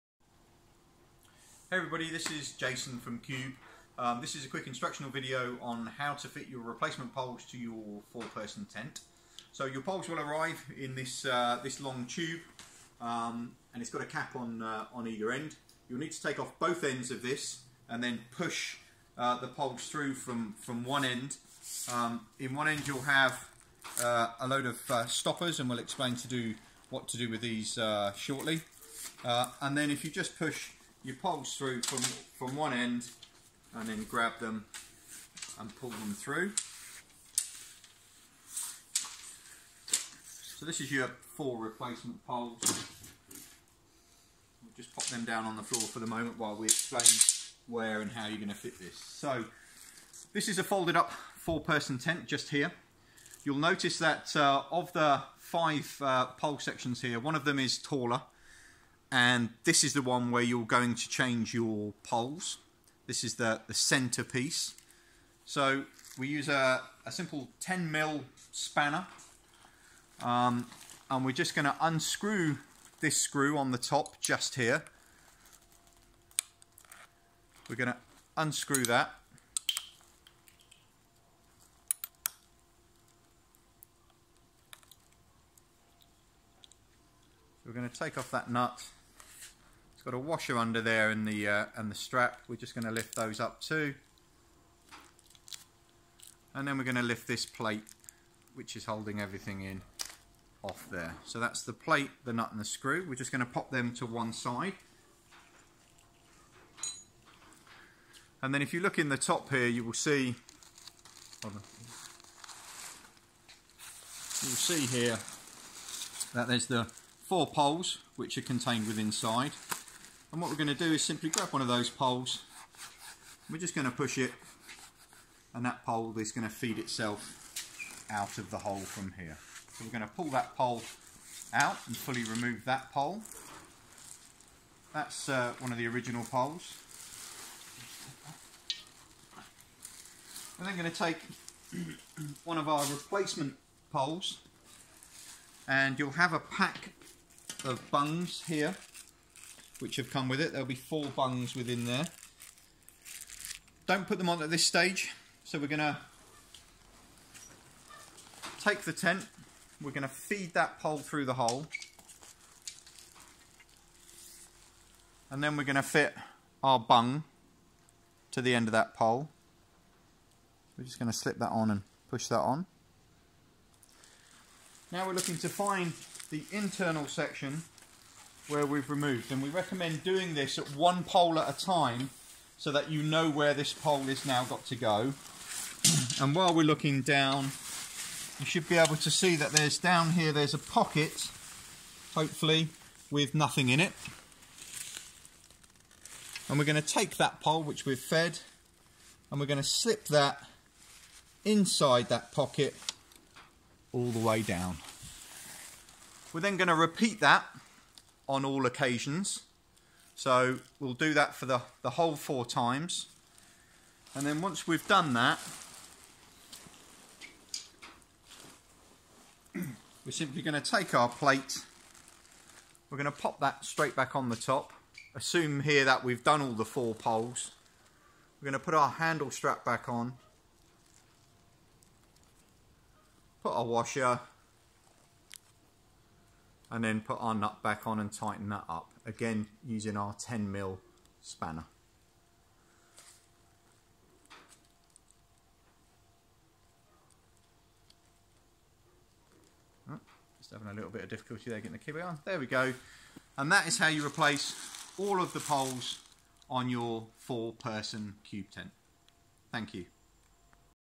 How to fit replacement poles on 4 person tent
How to fit your replacement poles in a 4 person Qube tent